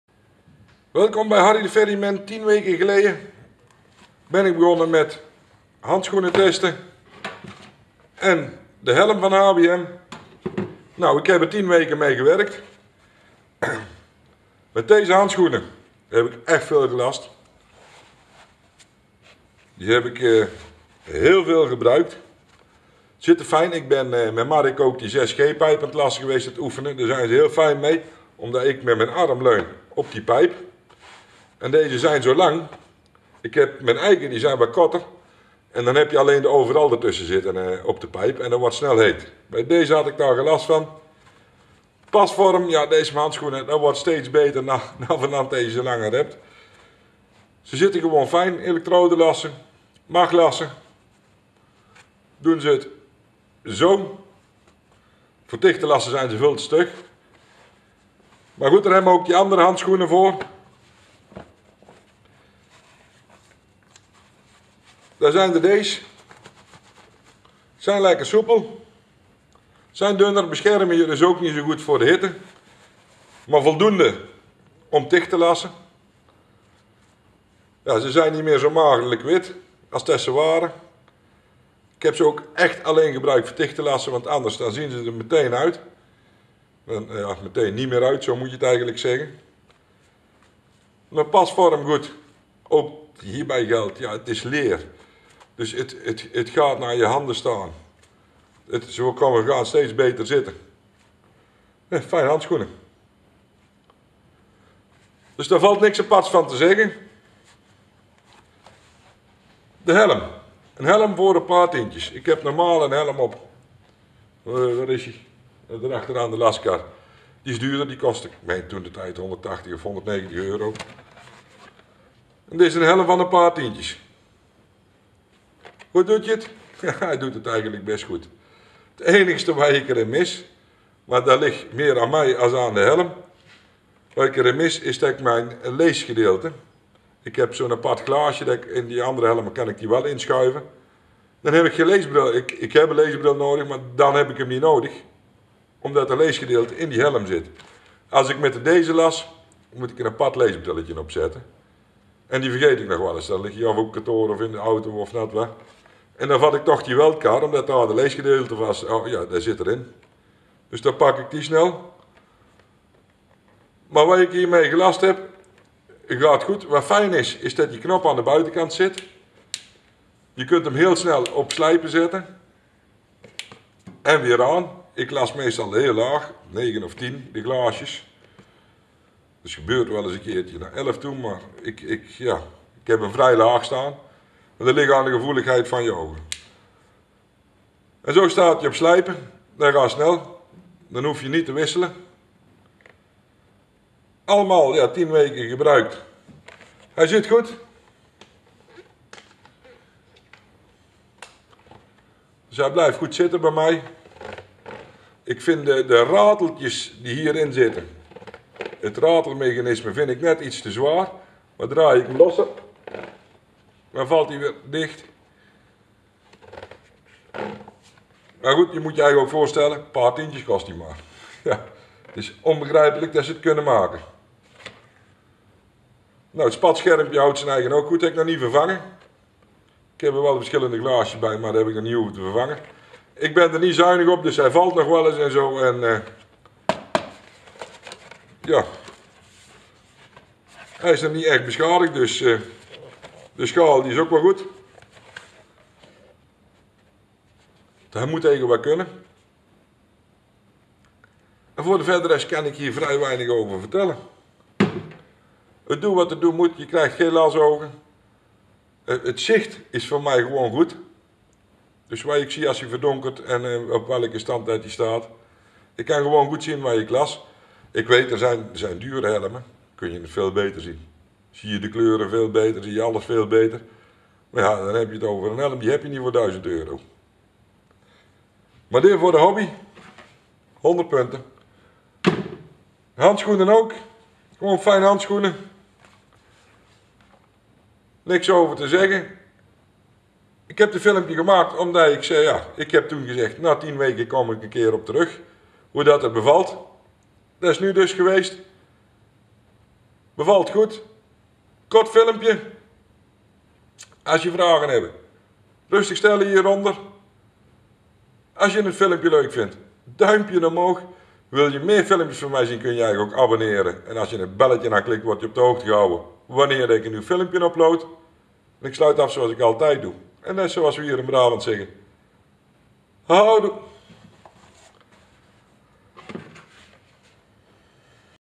De HBM lashelm en handschoenen hoe bevallen ze!!
De HBM lashelm en handschoenen hoe bevallen ze na tien weken.
hier krijg je mijn antwoord .